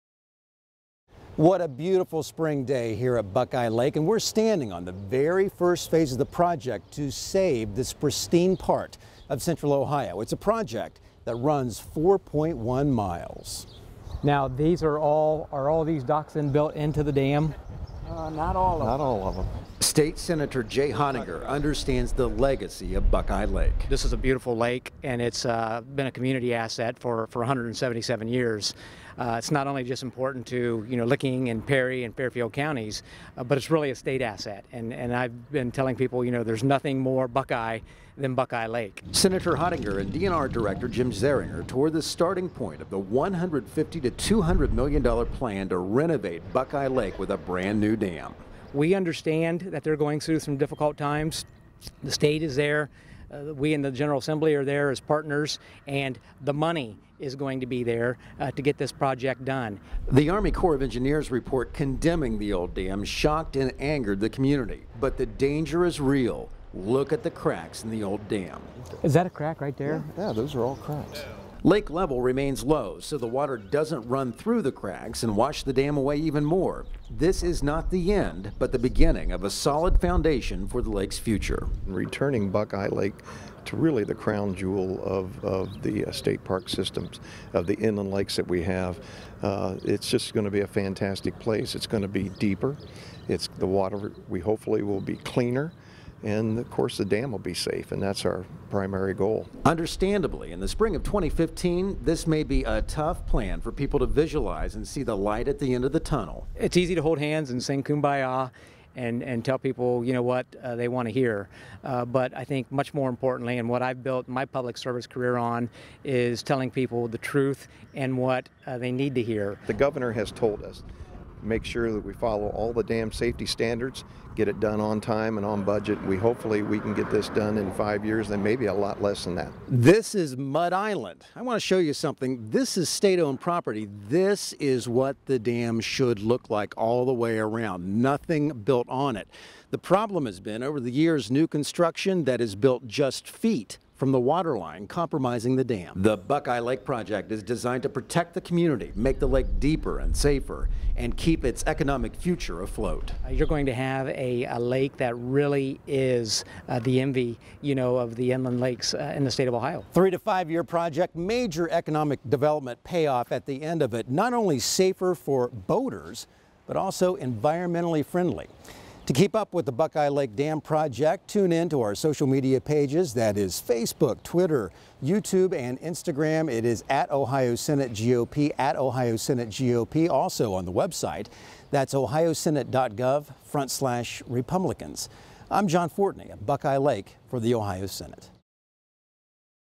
In Depth: Buckeye Lake Dam Renewal Project
Ohio State Senator Jay Hottinger and Ohio Department of Natural Resources  Director Jim Zehringer, tour the Buckeye Lake Dam renewal project. The dam was declared unsafe by the U.S.  Army Corps of Engineers and construction of a replacement $150 million dollar dam is slated to begin in the summer of 2016. Senator Hottinger highlights the efforts made by the Legislature, Governor's Office, and ODNR to return Buckeye Lake to its former status as one of Ohio's crown jewel inland lakes.